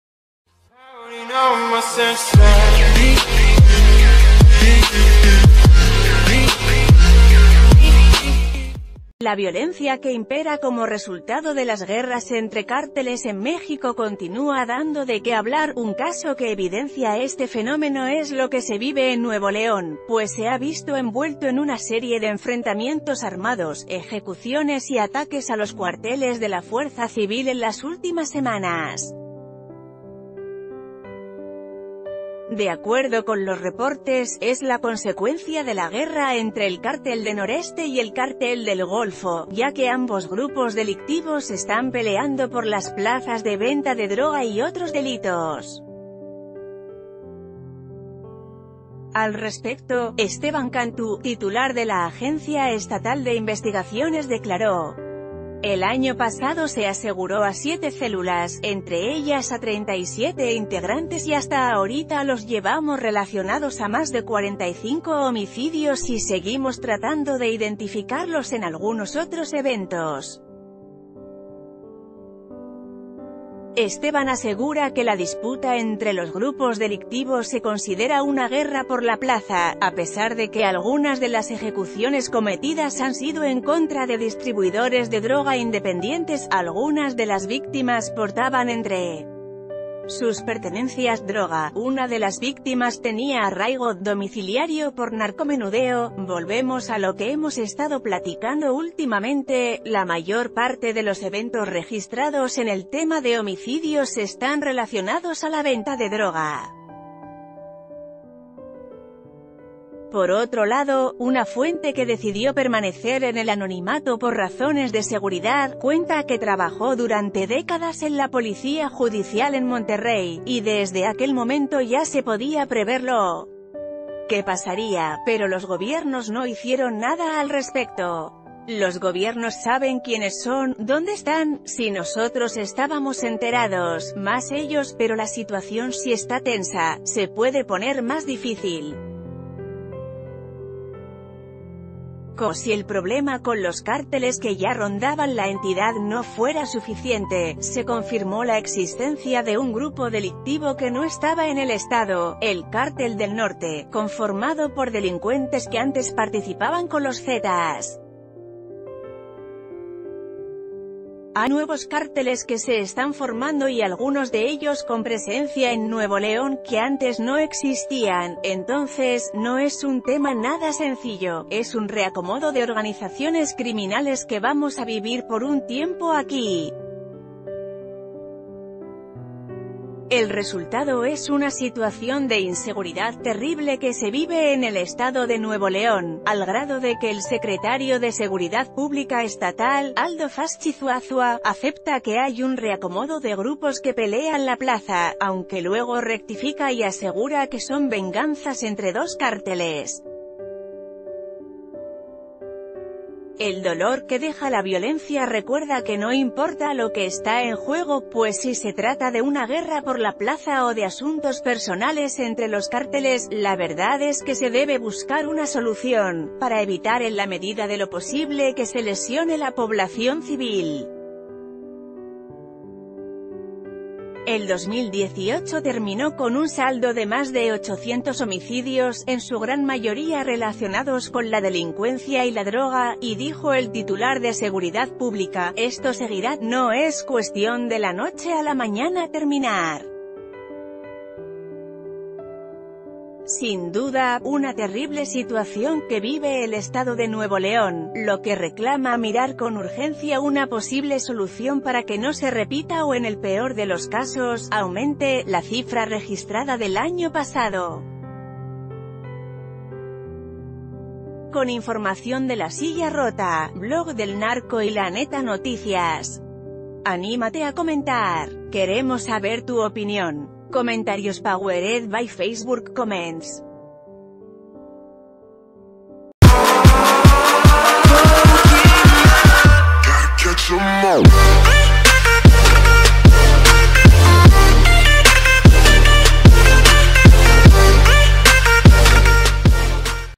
Cártel del Golfo y Cartel del Noreste: la sanguinaria guerra que libra Nuevo León
La violencia que impera como resultado de las guerras entre cárteles en México continúa dando de qué hablar, un caso que evidencia este fenómeno es lo que se vive en Nuevo León, pues se ha visto envuelto en una serie de enfrentamientos armados, ejecuciones y ataques a los cuarteles de la Fuerza Civi...